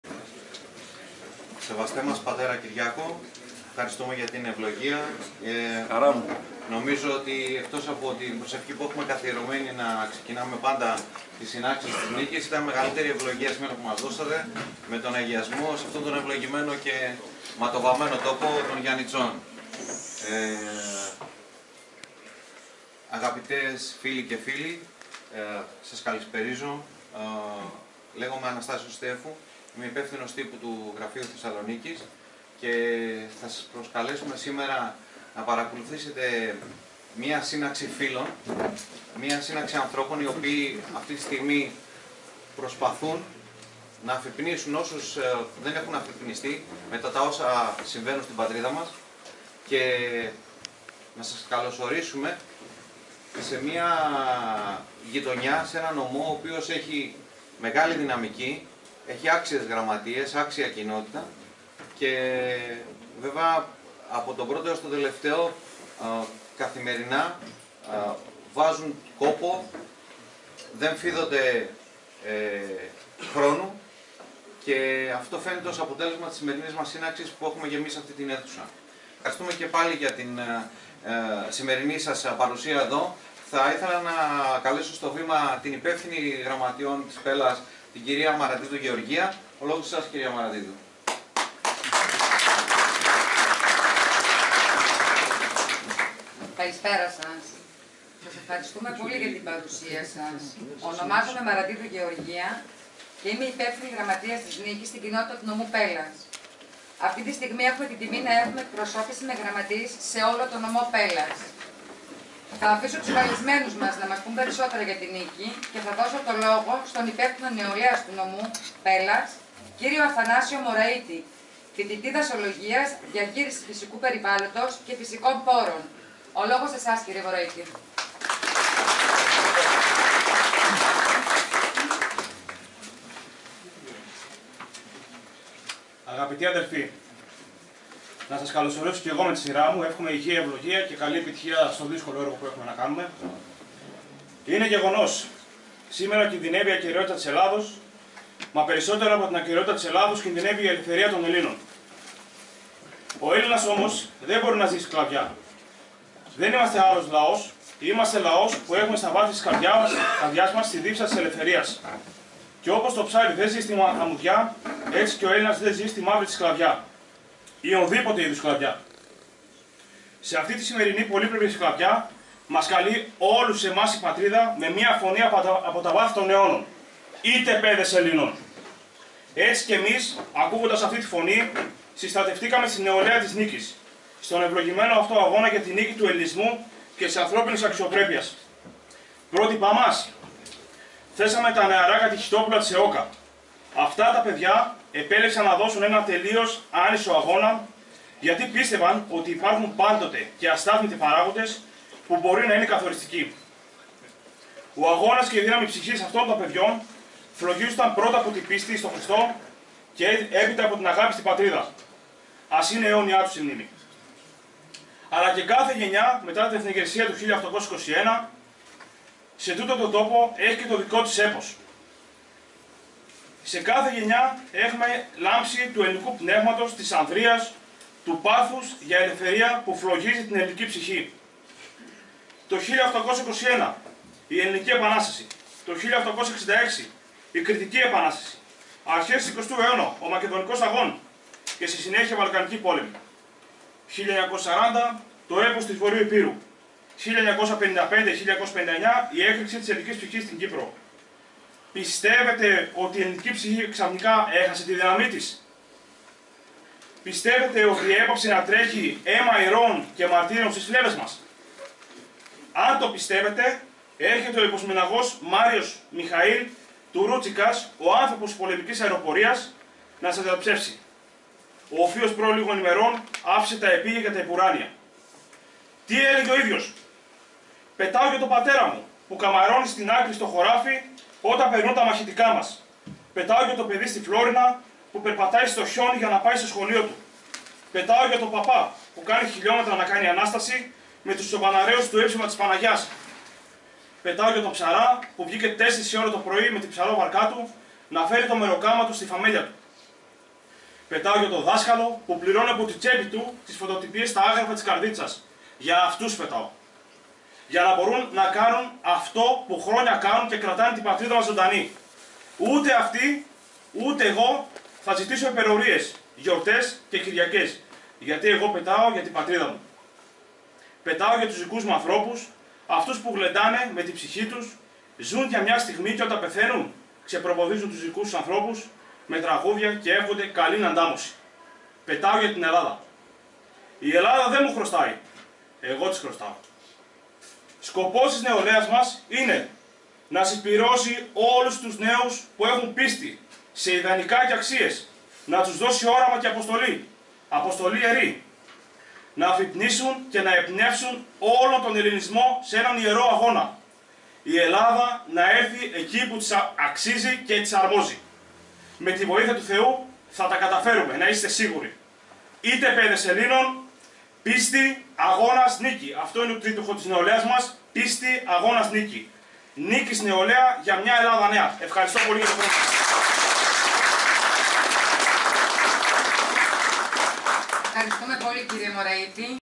Πίστη - Αγώνας - Νίκη | Νεολαία Πέλλας - Δημοκρατικό Πατριωτικό Κίνημα "ΝΙΚΗ" Γιαννιτσά 6/2/2023 (2)
Βίντεο:
Αντώνης Παπαδόπουλος
(Συμβαίνει Τώρα)
Δευτέρα 6/2/2023

"ΝΙΚΗ"
Δημοκρατικό Πατριωτικό Κίνημα
Πρόεδρος κ. Δημήτριος Νατσιός

Ανοιχτή Εκδήλωση στα Γιαννιτσά

Αίθουσα Εκδηλώσεων
Ξενοδοχείου "Πέλλα"